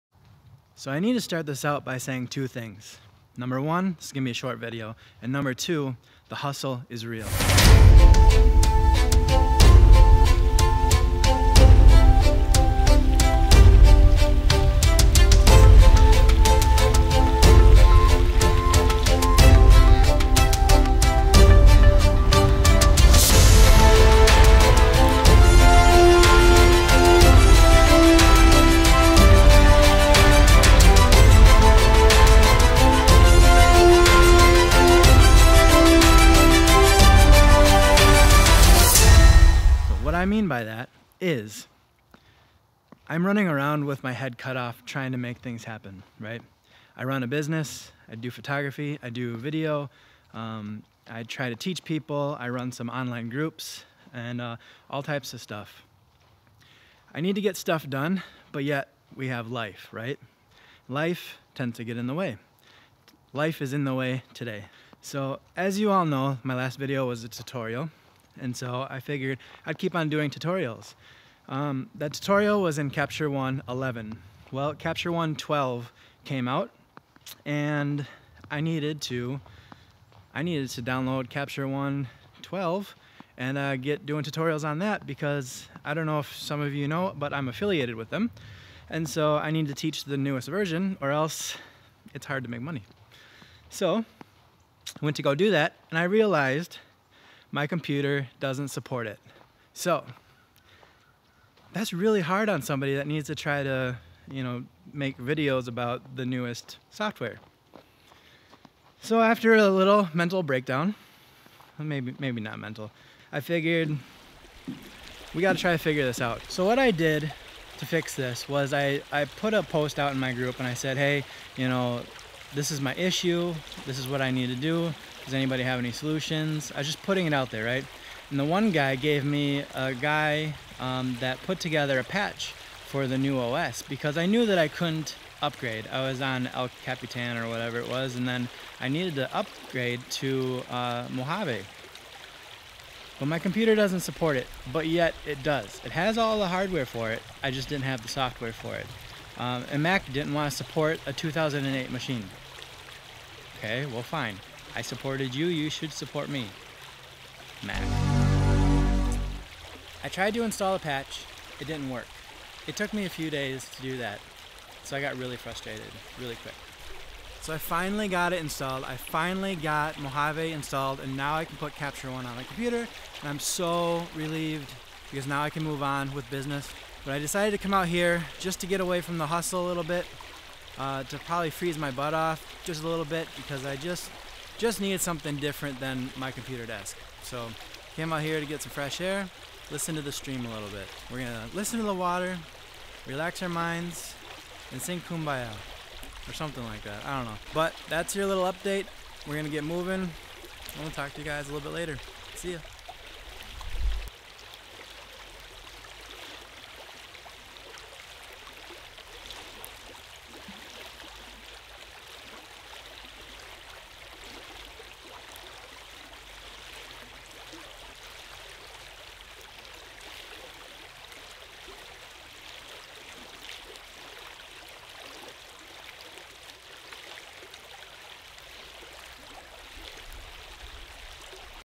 Let Me Explain...Mojave Patch, Mac pro 3,1 patch, Life patch!
I'm not sure if I explained this properly. But basically Apple won't support older macs and since I can't afford a new computer I feel screwed over. Another part of this is they also funnel you into buying more and more!  Since my work is partly working with new editing software. I started to feel frantic. not being able to pay my bills will force a major set back.
This dude saved my life! I know the time is still coming to buy a new computer but for now, we are good!

I used this patch to upgrade for El Capitan to Mojave on a Mac Pro 3,1 machine. It runs fast and smooth. Apple is just trying to make more money by funneling technology.